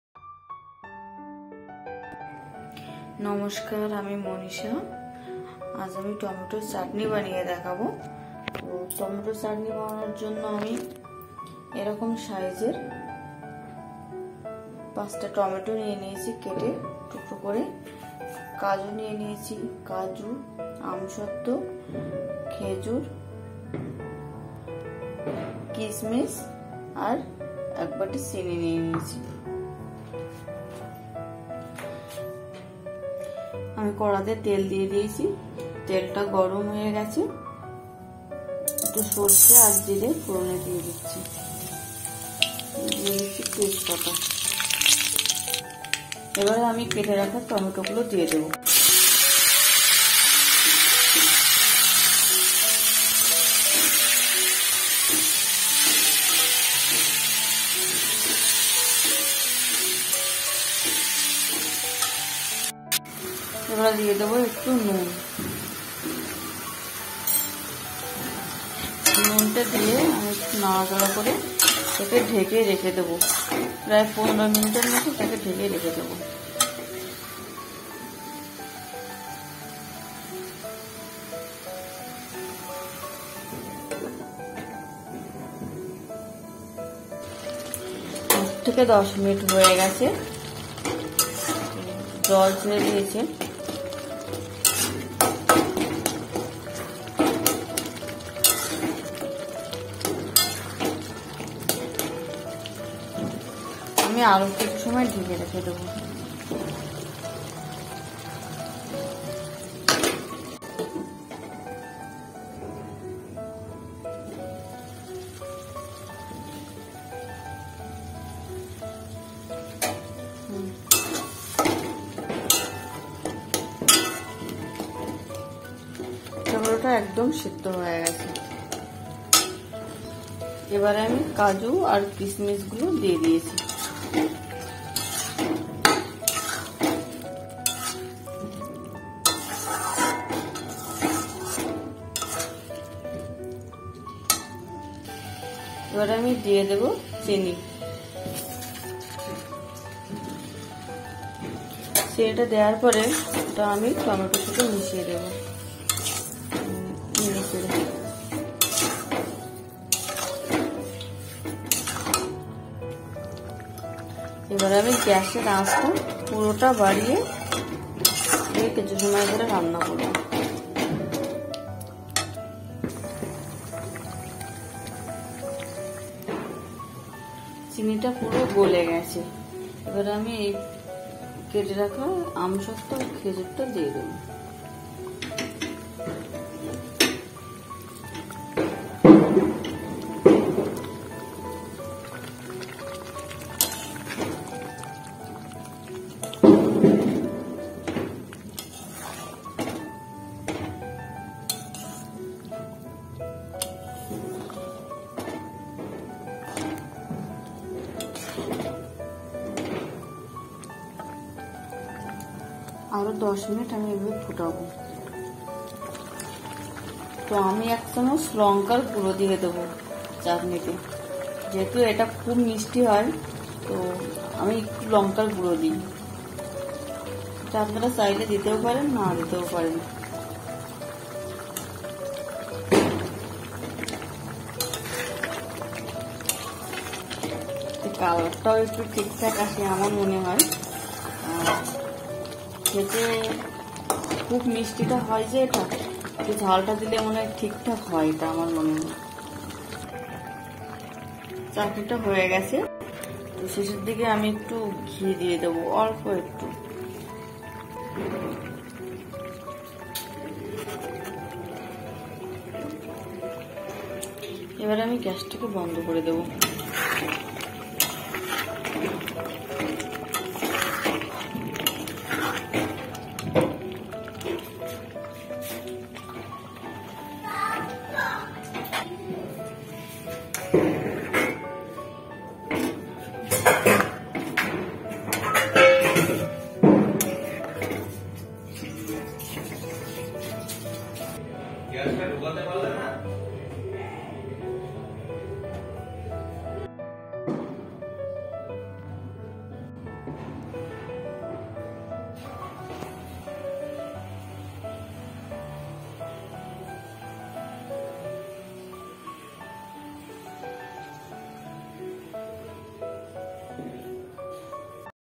How To Make Tomato Chutney Recipe!! টমেটোর চাটনি তৈরি সহজ পদ্ধতি।।।
guys hope you like this video for bengali sweet tomato chutney.

tomato5.
1tsp mustard
oil
1cup sugur.
salt
1tsp foron
.kaju
kichmish
1tsp red chili powder
mango papad